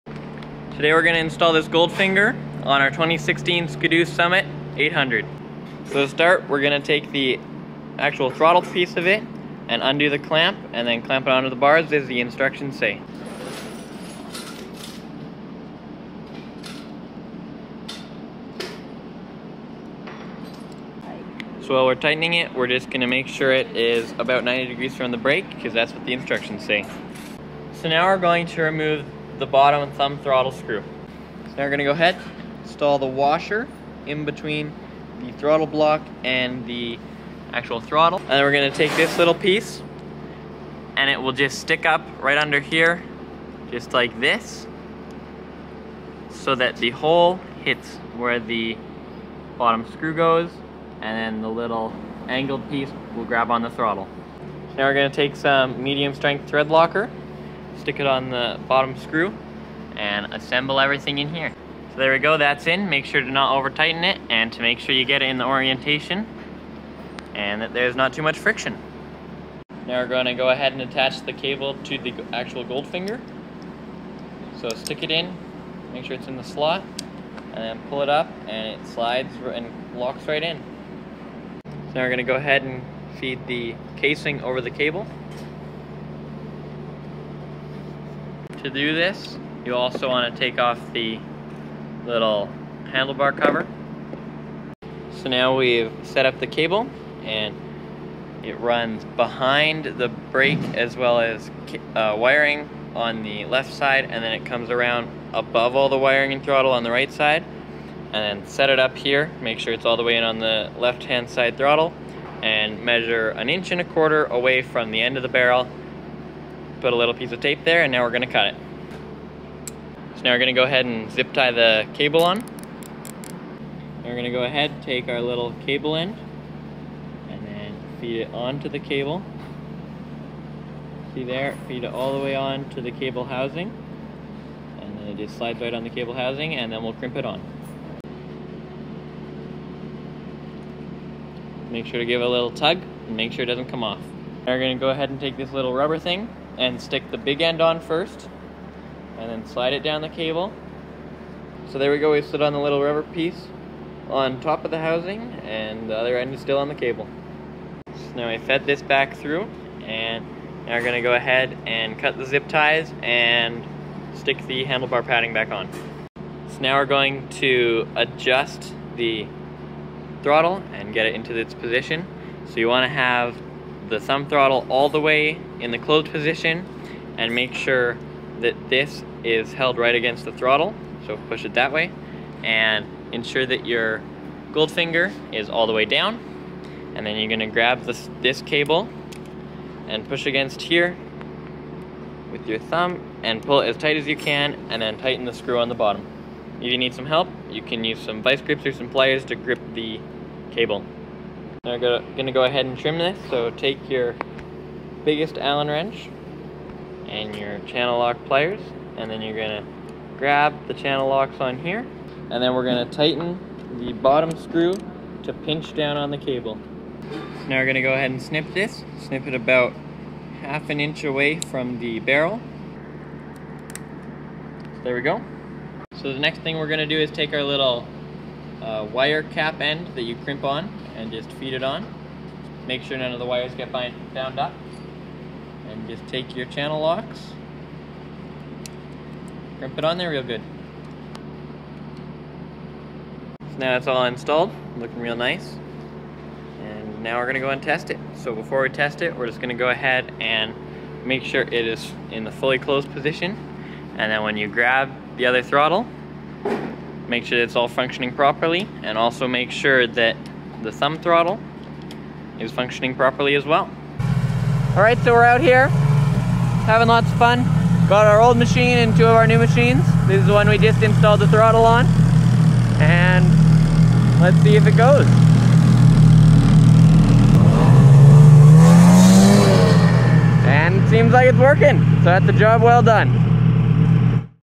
Installing A Gold Finger Left Hand Throttle On A 2016 Summit 800
Be Carful when installing the actual gold finger clamp as the screw heads easily strip out and become circles.